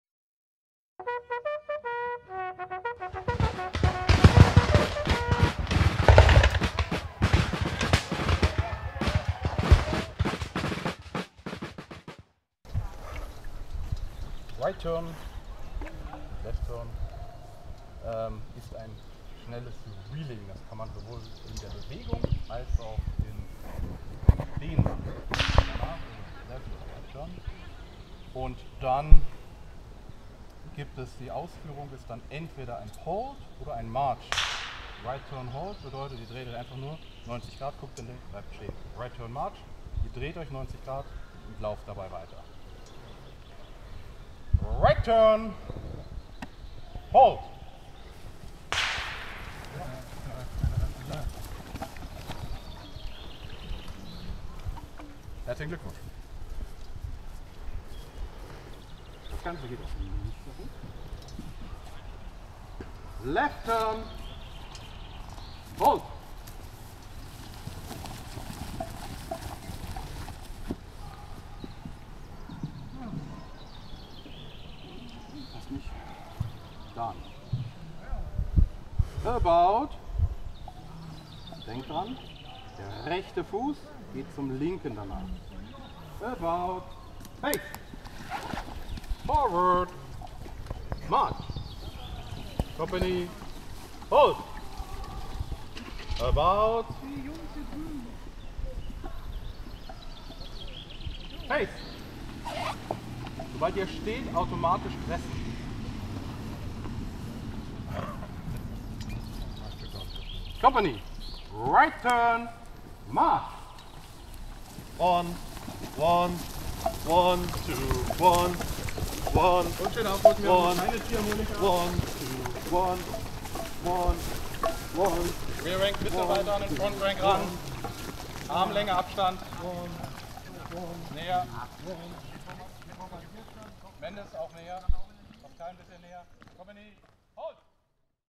Civil War Reenactment | Drehungen im Drill der 52ndNY | Luttmersen 2024
Ein weiterer Auszug aus dem ersten Drill der Einheit. Geübt wird das sofortige Drehen der Linie um 90°